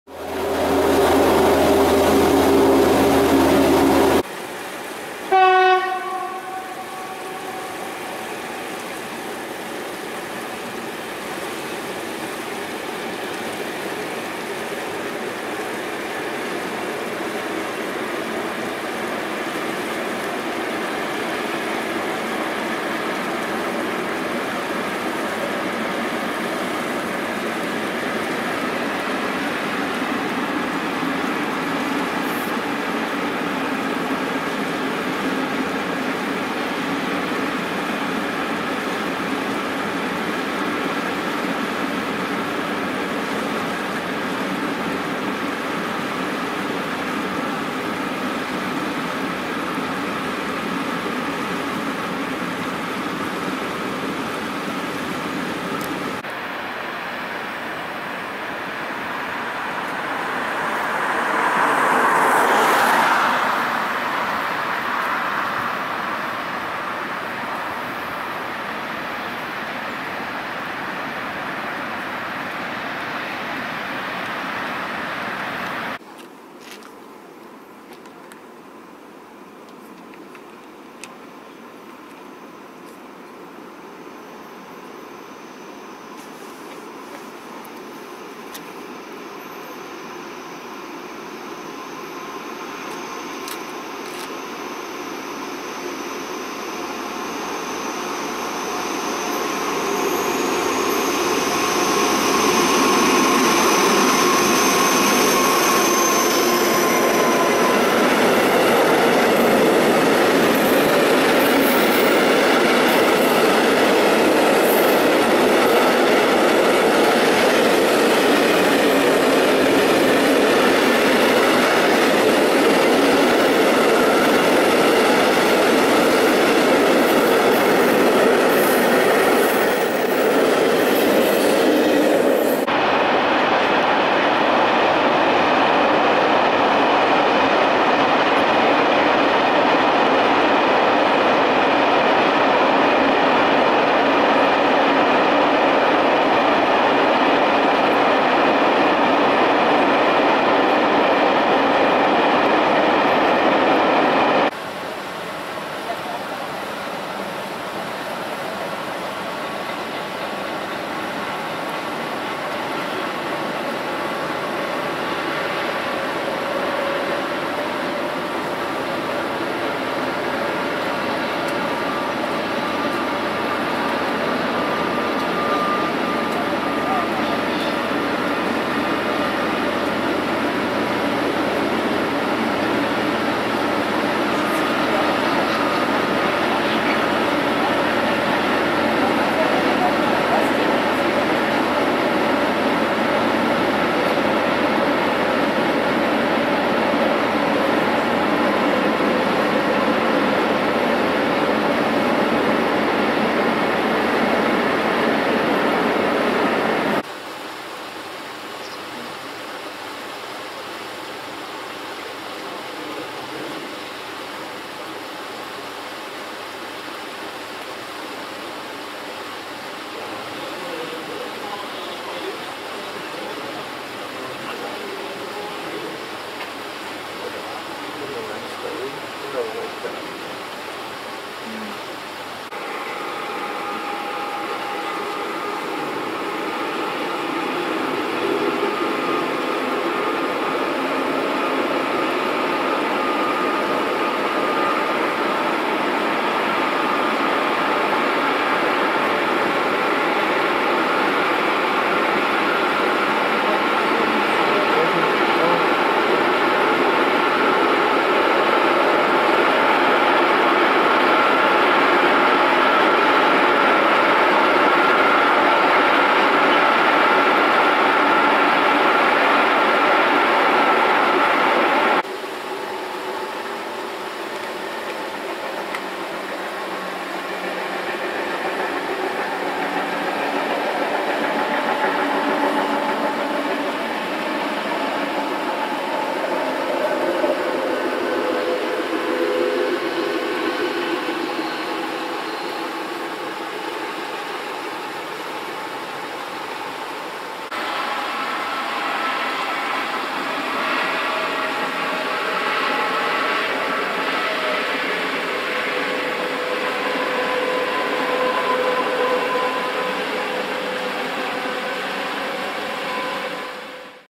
Güterzüge an der Elbe bei Bad Schandau
Dieser Film zeigt euch Güterzüge an der Elbe.